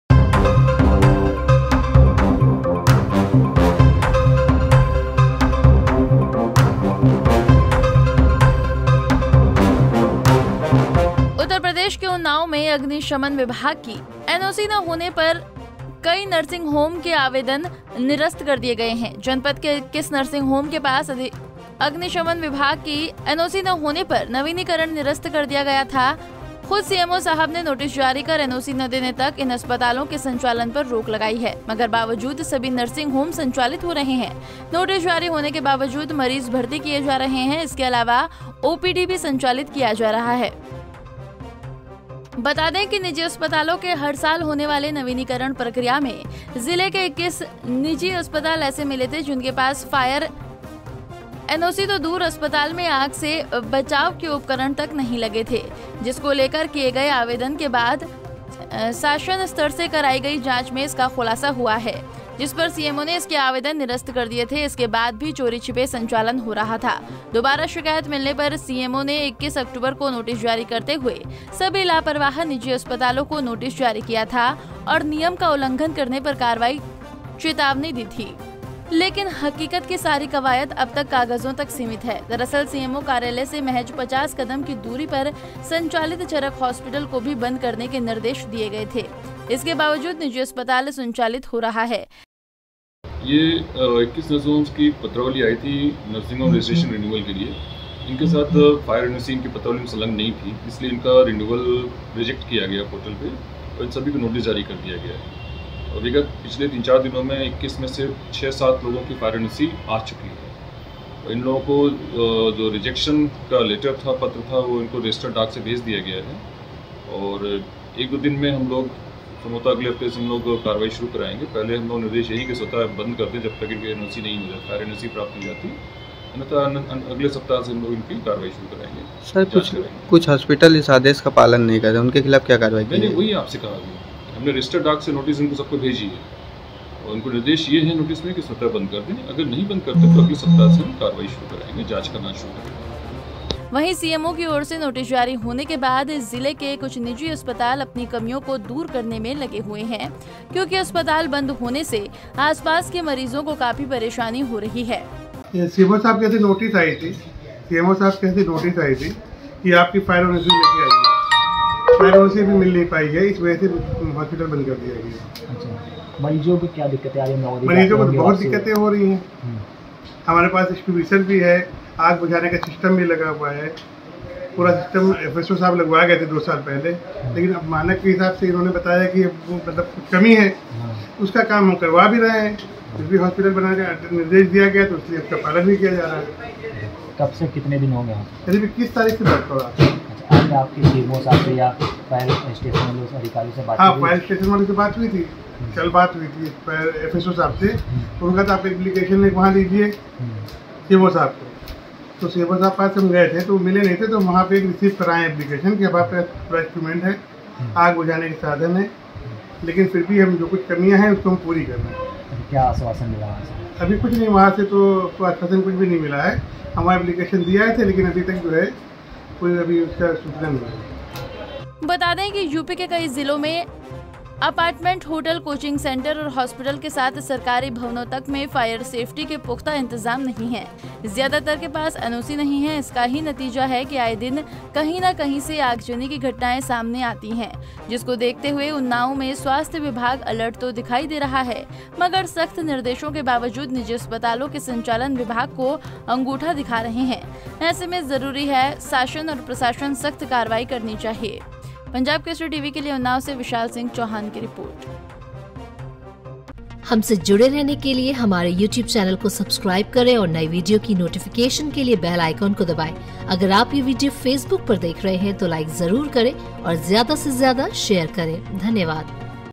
उन्नाव के 21 निजी अस्पतालों में फायर सेफ्टी की कमी, नोटिस के बावजूद हो रहा संचालित
उत्तर प्रदेश के उन्नाव में अग्निशमन विभाग की एनओसी न होने पर में कई नर्सिंग होम के आवेदन निरस्त कर दिये गये हैं... जनपद के 21 नर्सिंग होम के पास अग्निशमन विभाग की एनओसी न होने पर नवीनीकरण निरस्त कर दिया गया था...खुद सीएमओ साहब ने नोटिस जारी कर एनओसी न देने तक इन अस्पतालों के संचालन पर रोक लगाई है...मगर, बावजूद  सभी नर्सिंग होम संचालित हो रहे हैं...नोटिस जारी होने के बावजूद मरीज भर्ती किए जा रहे हैं...इसके अलावा ओपीडी भी संचालित किया जा रहा है...